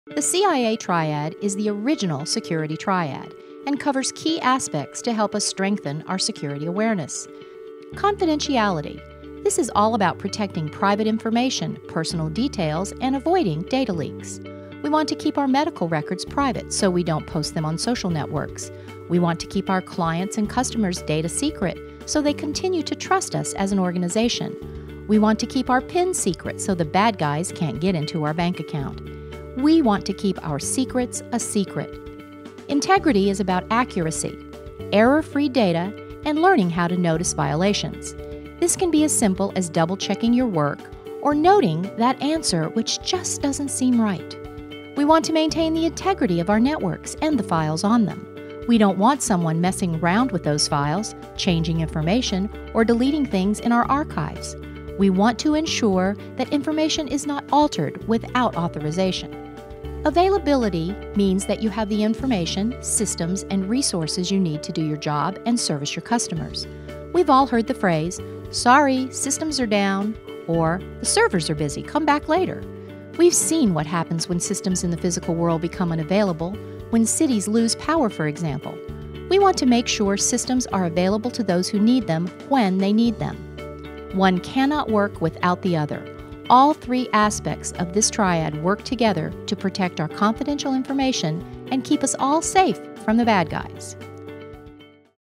Security Express: The CIA Triad
The CIA triad is one of the security triads, a well-known model for security policy development, used to identify problem areas and necessary solutions for information security. Confidentiality, Integrity,and Availability are the cornerstones of successful security awareness.